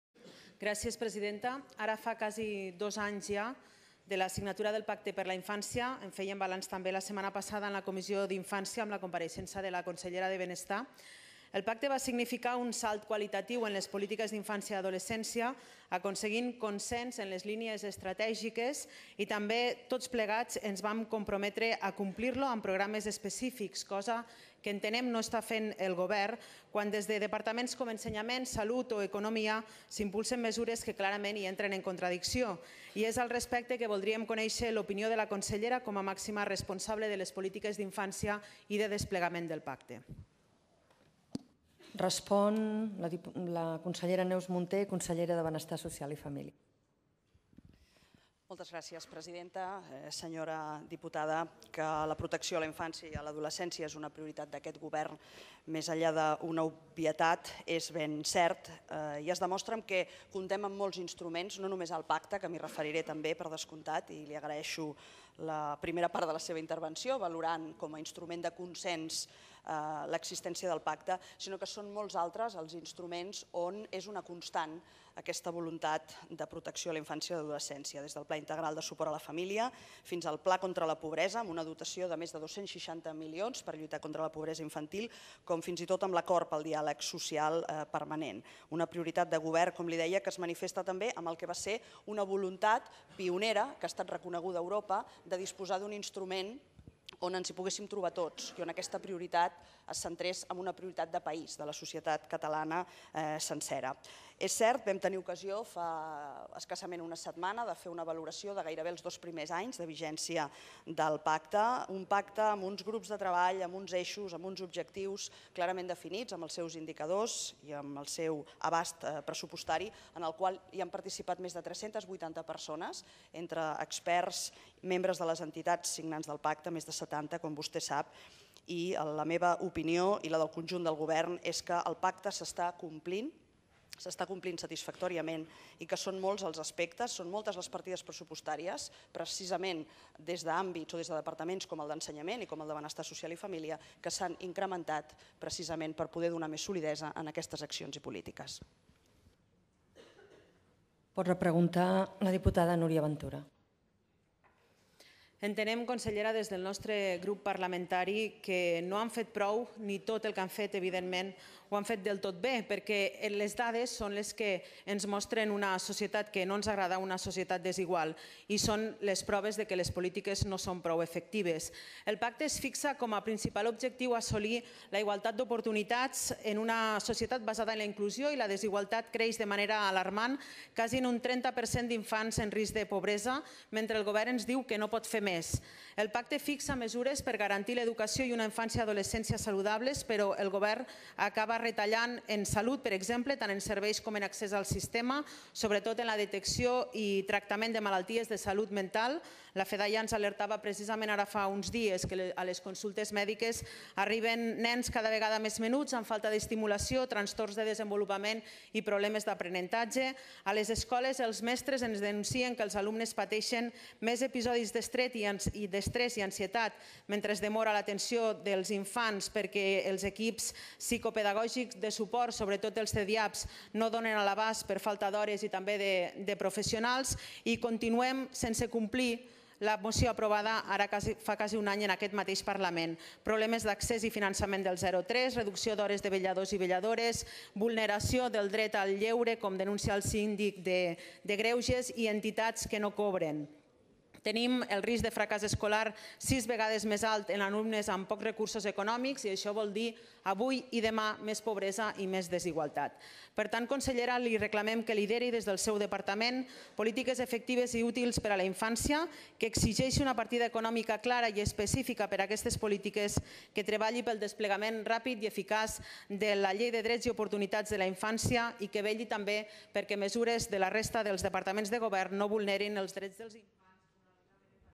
Mancances en les polítiques d’infància
Núria Ventura ha denunciat, en la sessió de control al Govern del Ple del Parlament, les mancances en les polítiques d’infància “No han fet prou i ni tot el que han fet ha estat adequat. Les dades ens mostren una societat que no ens agrada, una societat desigual, prova evident de que les polítiques no són prou efectives.”

Núria Ventura ha afegit “La desigualtat creix a un ritme alarmant, assolint un 33% d’infants en risc de pobresa mentre el Govern ens diu que no pot fer més. I aquests alumnes tenen sis vegades més probabilitats de fracás escolar, fent que la pobresa d’avui també ho sigui de futur.”